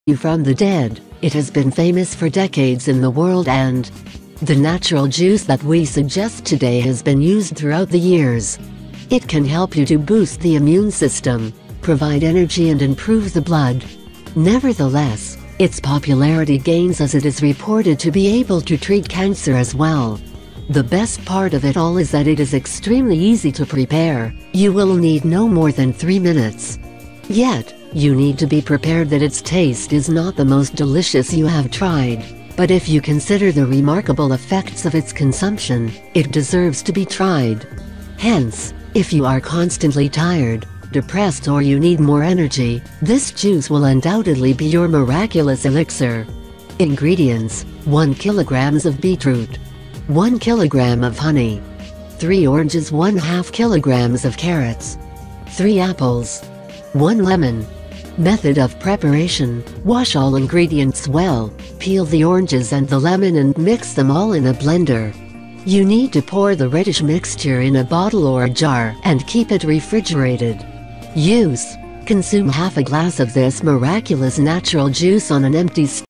A JUICE WHICH WILL RAISE YOU FROM THE DEAD It Has Been Famous For Decades In The World
A JUICE WHICH WILL RAISE YOU FROM THE DEAD It Has Been Famous For Decades In The World And
2 Drops Of This In Your Ears And 97 Of Your Hearing Recovers! Even Old People From 80 To 90 Are D